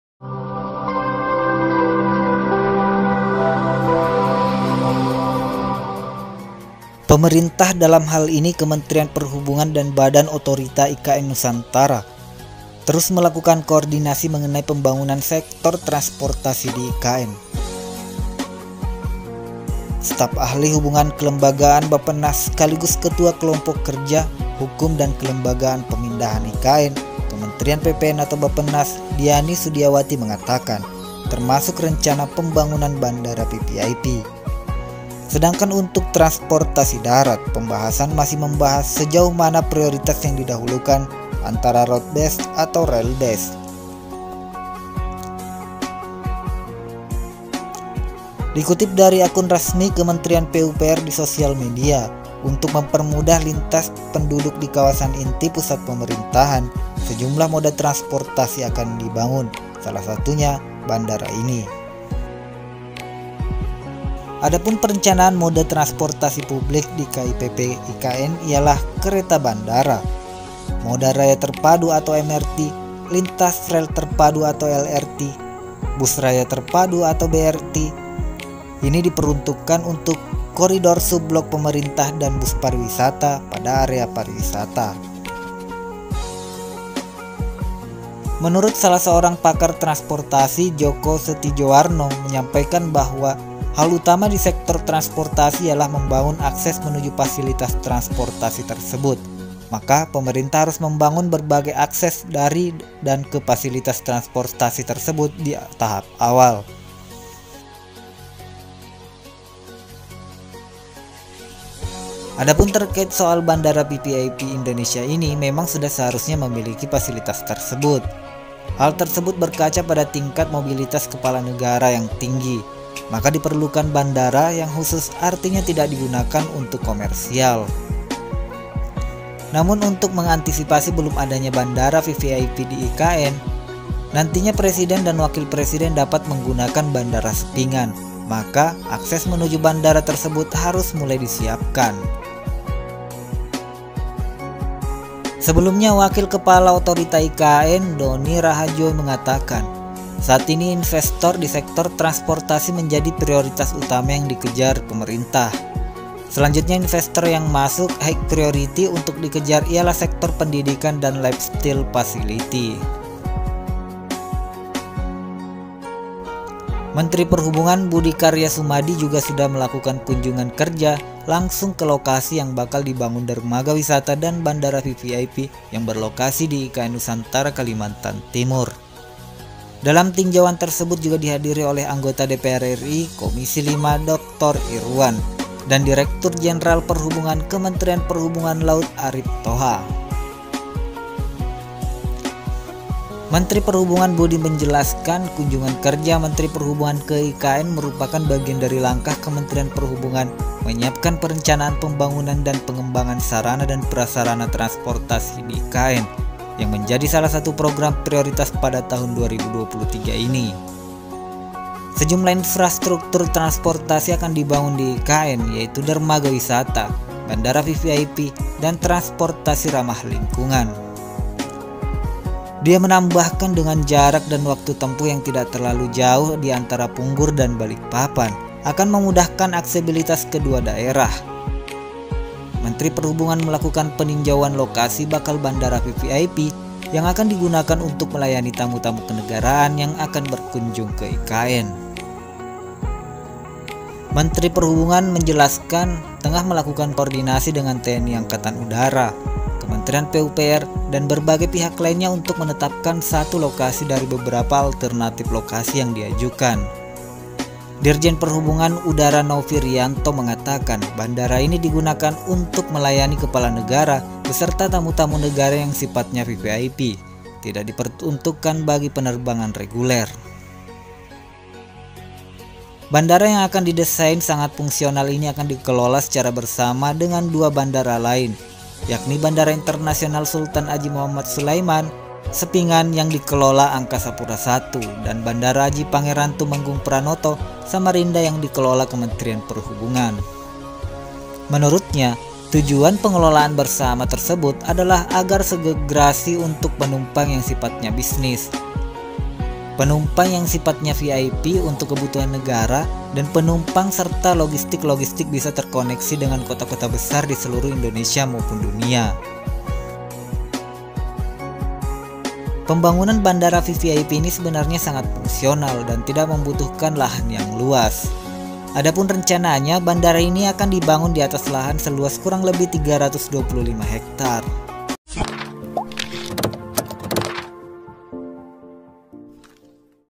IKN HARI INI‼️Akan Dibangun Bandara VVIP di IKN Khusus Kepala Negara dan Tamu Penting
Pemerintah berencana akan membanguncbandara vvip di kawasan IKN.

Menhub Budi menjelaskan, kunjungan kerja Menhub ke IKN merupakan bagian dari langkah Kemenhub menyiapkan perencanaan pembangunan dan pengembangan sarana dan prasarana transportasi di IKN yang menjadi salah satu program prioritas pada tahun 2023.

Dirjen Perhubungan Udara Novie Riyanto mengatakan bandara ini digunakan untuk melayani Kepala Negara beserta tamu-tamu negara yang sifatnya VVIP. Tidak diperuntukkan bagi penerbangan reguler.